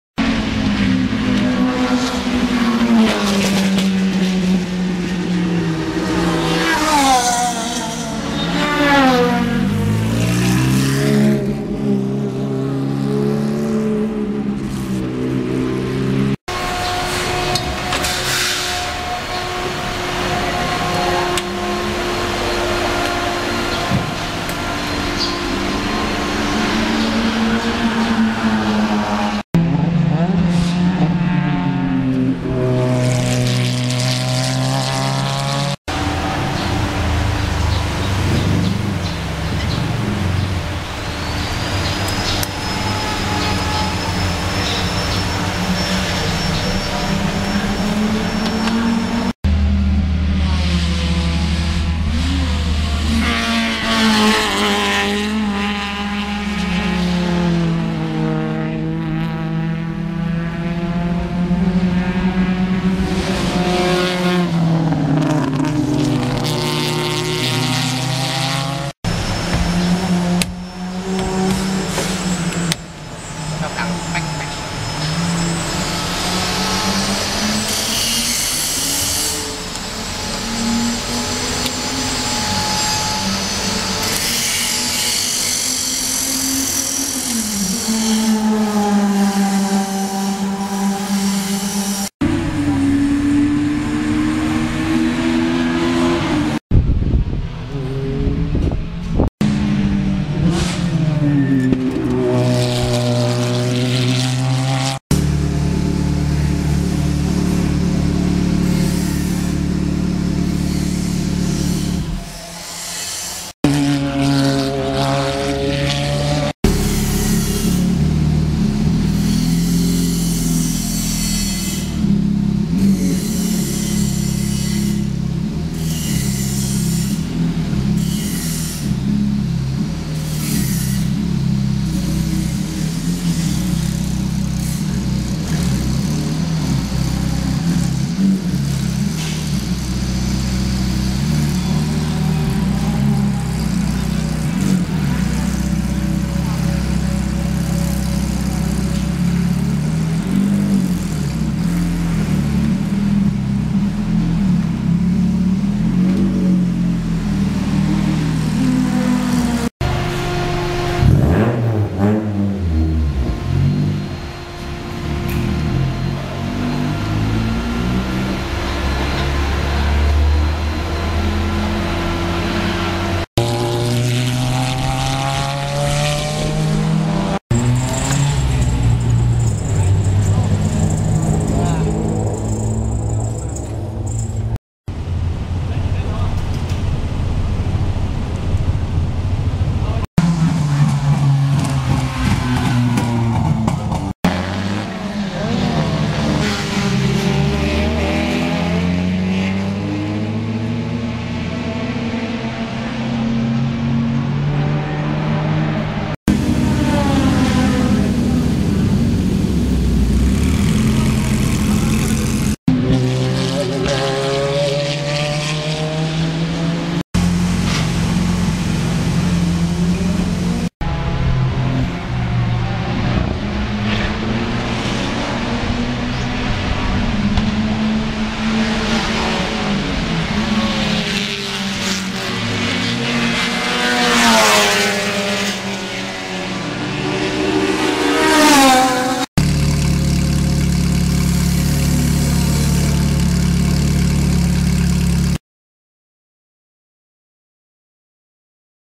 K&M Competizione Ferrari F430 GT3 Test @ Bira International Circuit (15 Jun 2012)
Tanat Thanakitamnuay tests the K&M Competizione Ferrari F430 GT3 at Bira International Circuit ahead of its Super Car Thailand 2012 debut at Sepang Circuit next month (15 Jun 2012).